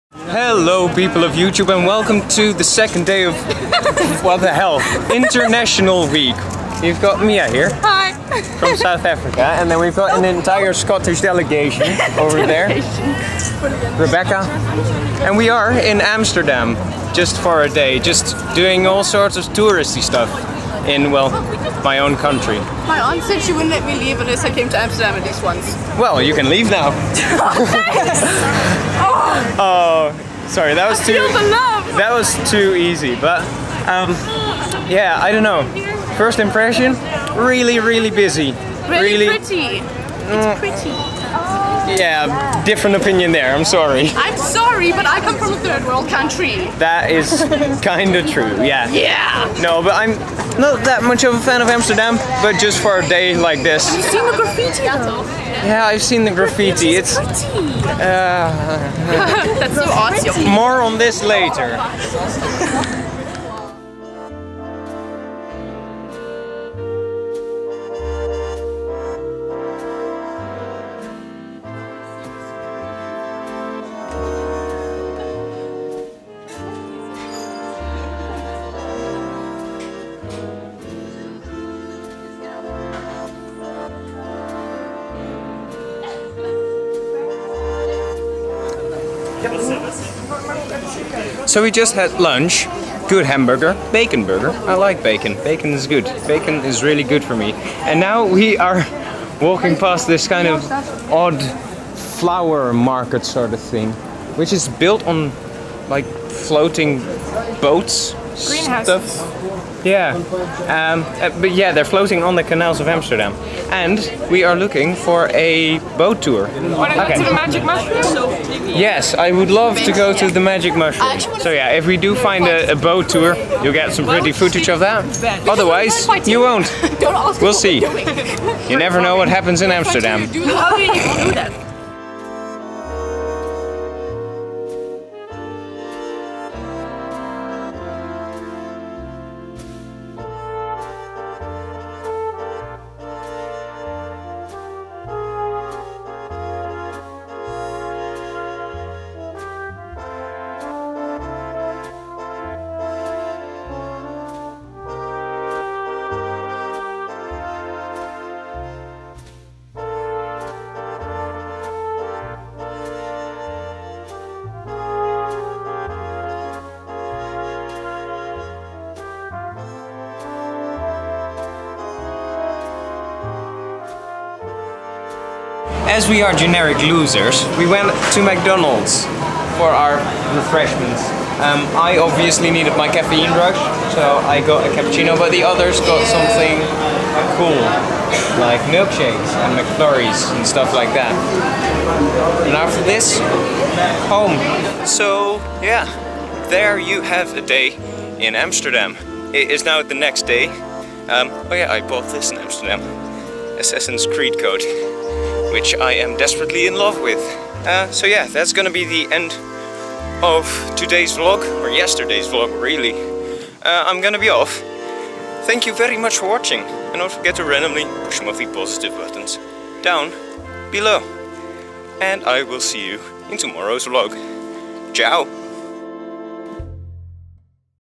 International Week - Day 2 | Amsterdam
Ft. slow music, a severe lack of weed and many a frowning face